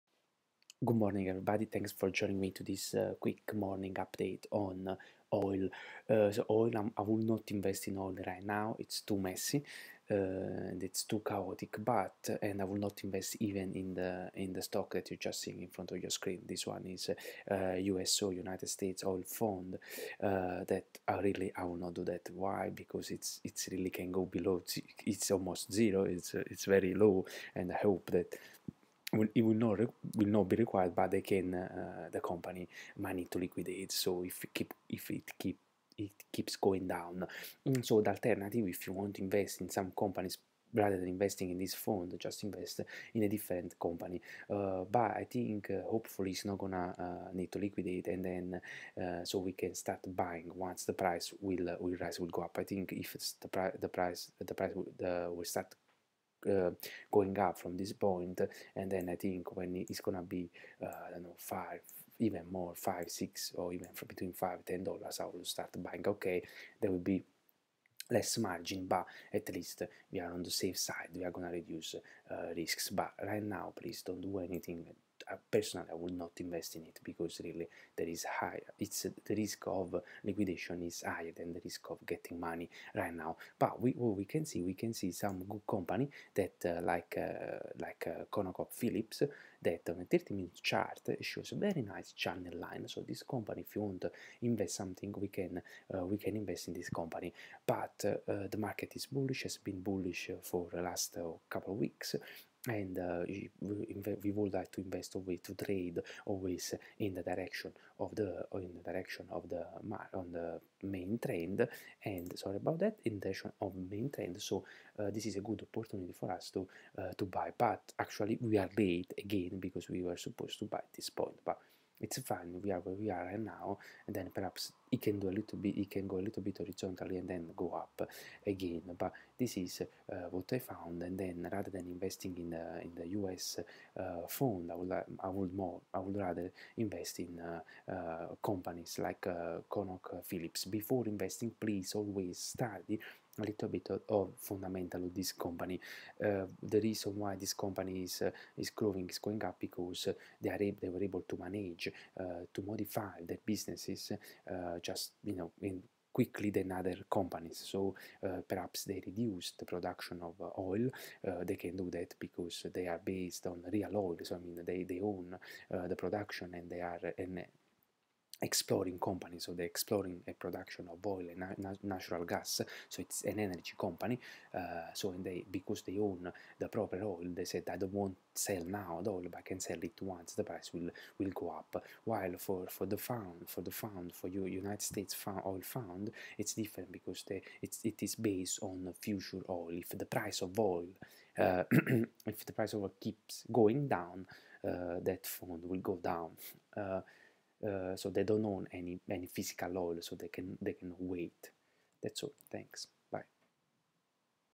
USO - US Oil Fund Stock today 28/04/2020 and ConocoPhillips Stock Today - COP Stock - Channel Line
I would not invest right now in this Fund. Just Wait and Stand Aside
Short term Analysis does not give any significant signal. Perhaps, there might be a rally to get the bottom line (level) of the previous Descending triangle.

ConocoPhillips Stock

This Market has been bullish for the last week.
For a short term investment, the channel line shows a target price to be reached so we might want to go long